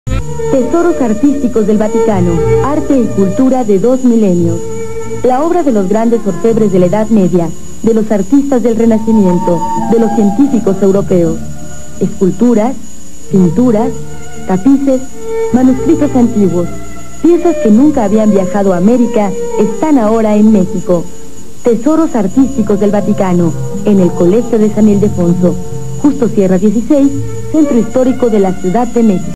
Comerciales mexicanos: CONACULTA 1994 7
Exposición Tesoros artísticos de Vaticano en compañía del Gobierno del Distrito Federal y la UNAM